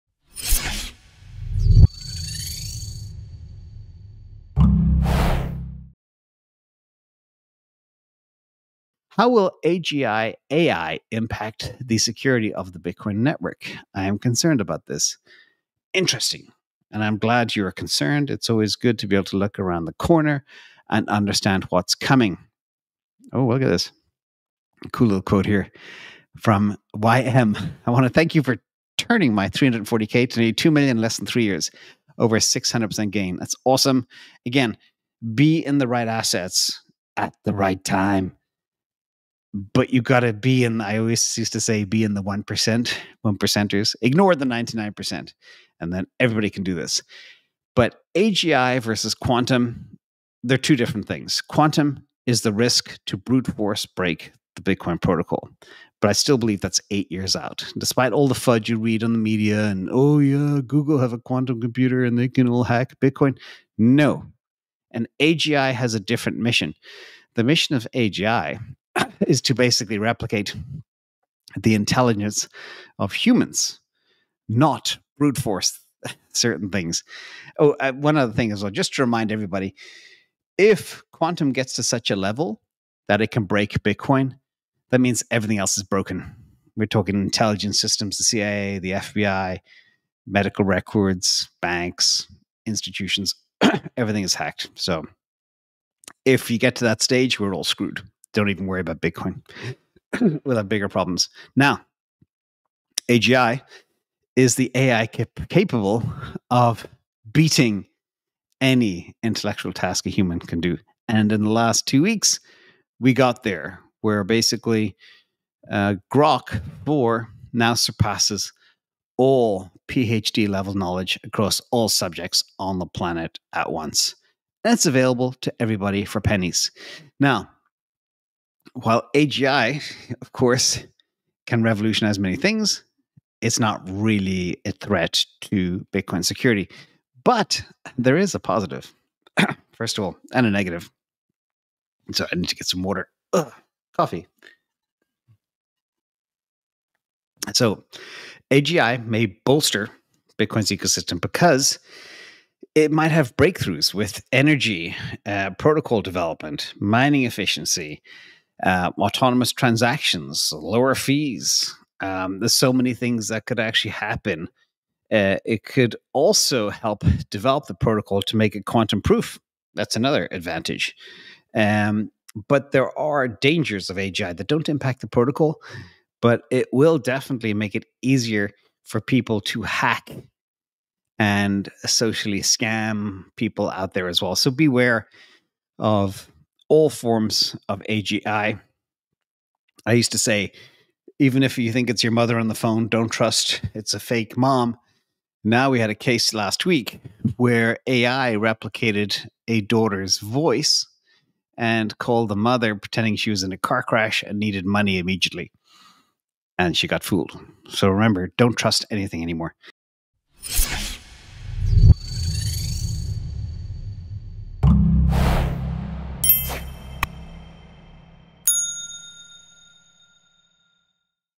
Will AI Threaten the Bitcoin Network?
00:00 Introduction and Concerns about AI Impact on Bitcoin
00:47 Understanding AGI and Quantum Computing
01:43 Potential Benefits and Risks of AGI